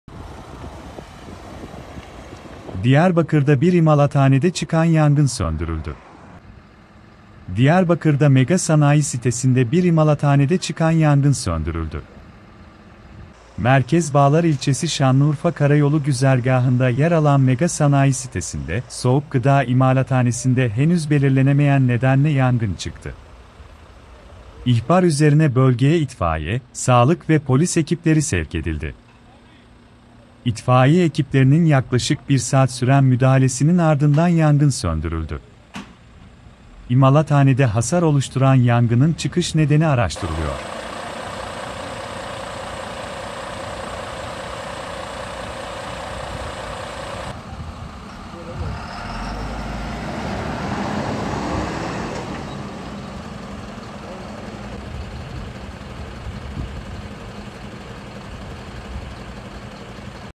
Diyarbakır'da bir imalathanede çıkan yangın söndürüldü
Diyarbakır'da Mega Sanayi Sitesi'nde bir imalathanede çıkan yangın söndürüldü.

Merkez Bağlar ilçesi Şanlıurfa kara yolu güzergahında yer alan Mega Sanayi Sitesi'nde soğuk gıda imalathanesinde henüz belirlenemeyen nedenle yangın çıktı.İhbar üzerine bölgeye itfaiye, sağlık ve polis ekipleri sevk edildi.

İtfaiye ekiplerinin yaklaşık bir saat süren müdahalesinin ardından yangın söndürüldü.İmalathanede hasar oluşturan yangının çıkış nedeni araştırılıyor.